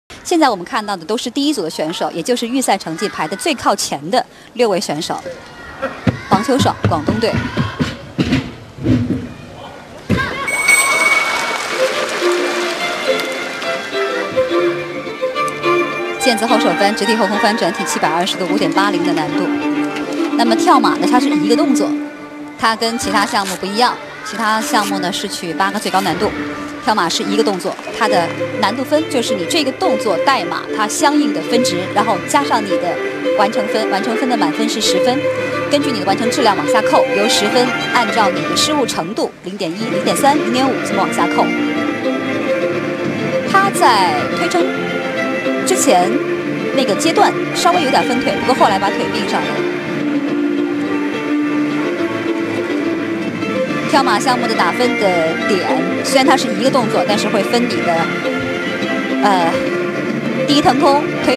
Huang Qiushuang 2012 Nationals AA VT (CCTV)
Huang Qiushuang 2012 Nationals AA VT, Score: 14.500